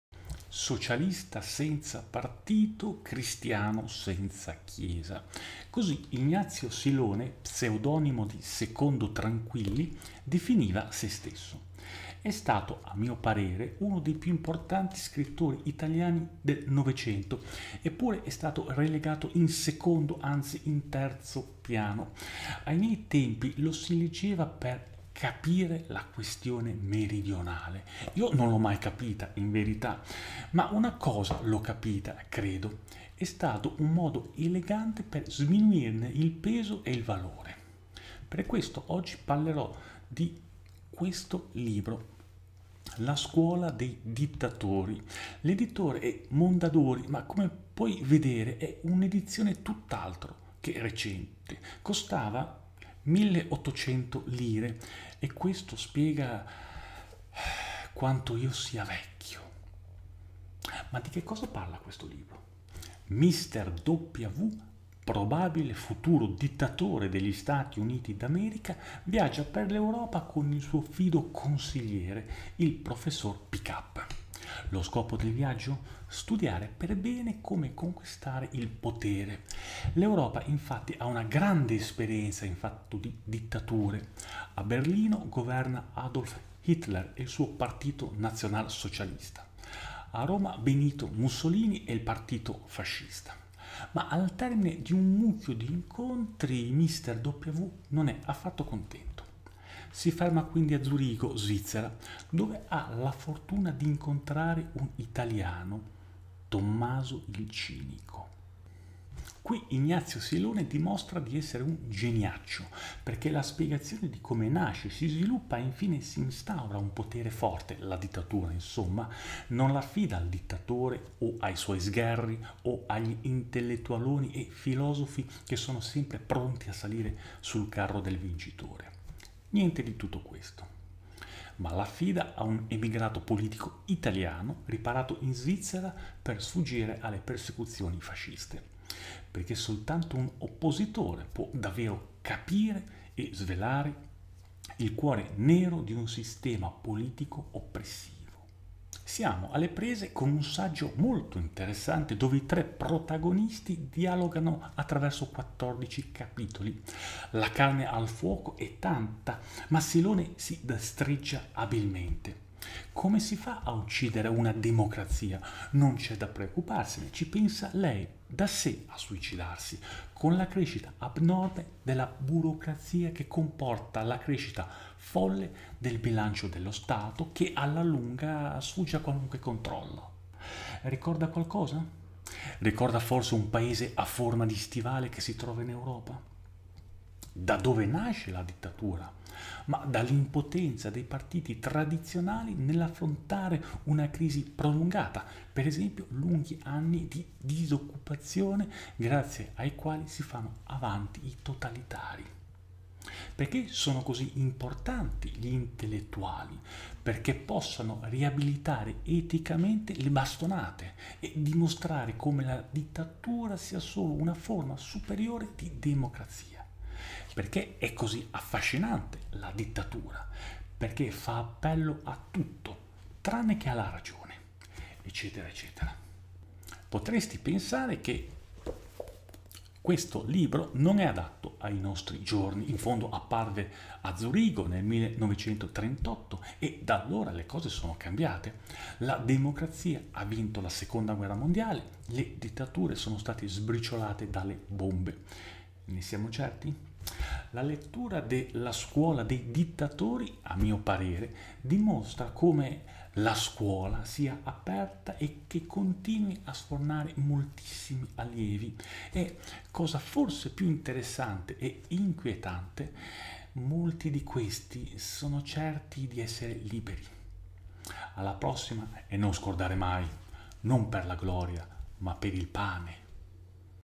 Consigli di lettura: La scuola dei dittatori di Ignazio Silone
Socialista senza partito. Cristiano senza Chiesa. Così Ignazio Silone, pseudonimo di Secondino Tranquilli, definiva se stesso. È stato a mio parere uno dei più importanti scrittori italiani del Novecento, eppure è stato relegato in secondo, anzi, in terzo piano. Ai miei tempi lo si leggeva per “capire la questione meridionale”. Io non l’ho mai capita, in verità, ma una cosa l’ho capita, credo...